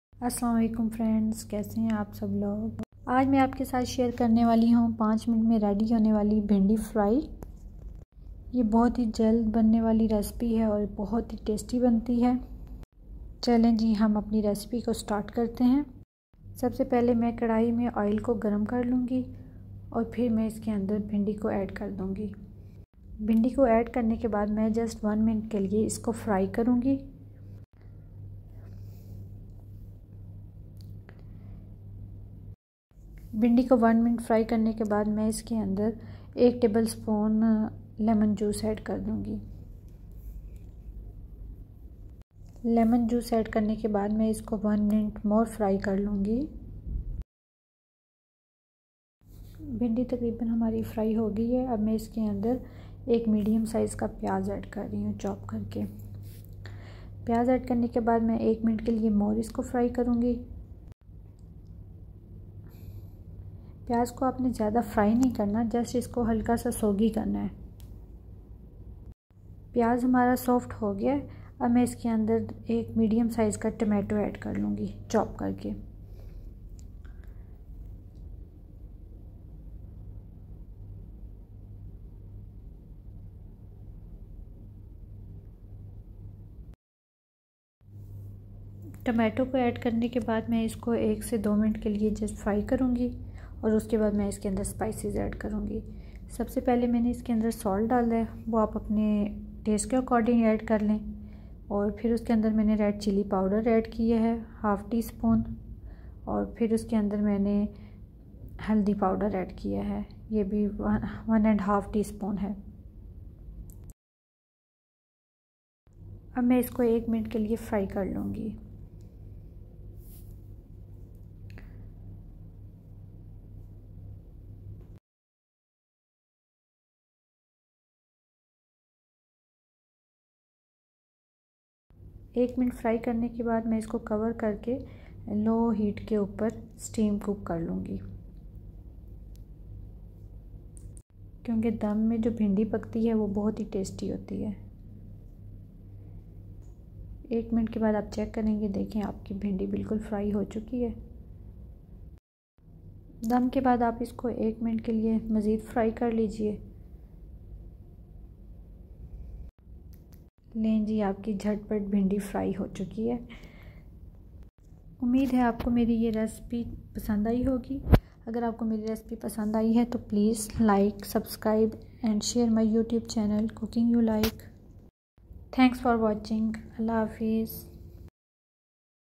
5 minutes Bhindi fry recipe#CookingYouLike
Today recipe is bhindi fry with simple ingredients. Hope you all like my recipe. yummy bhindi fry

Ingredients:-

Bhindi 250 grm
Oil